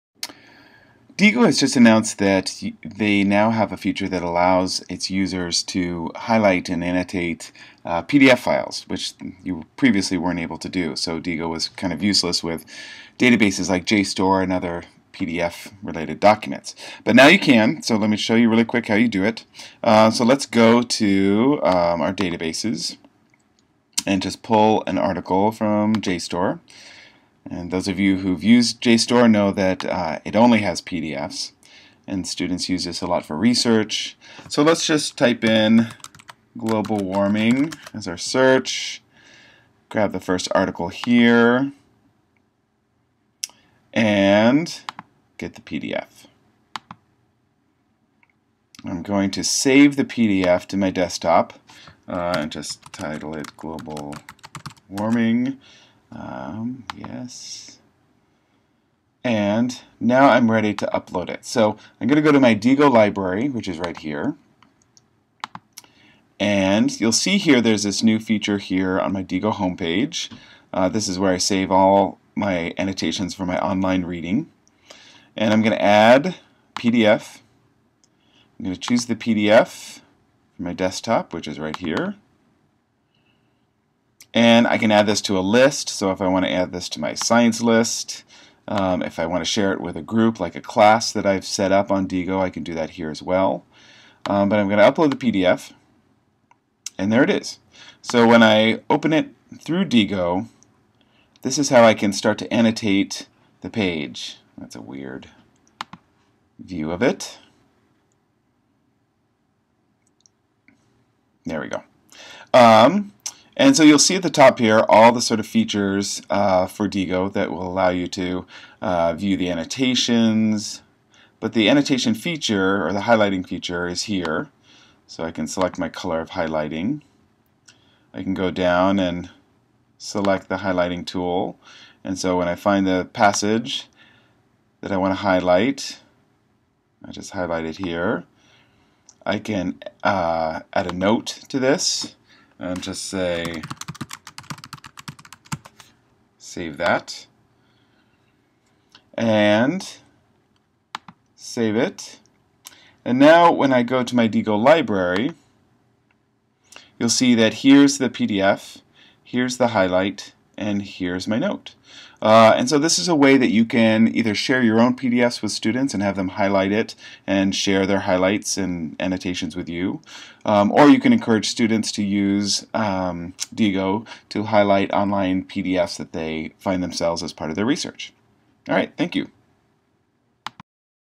Annotating pdfs in Diigo
Now you can annotate pdfs in Diigo.  Here's how.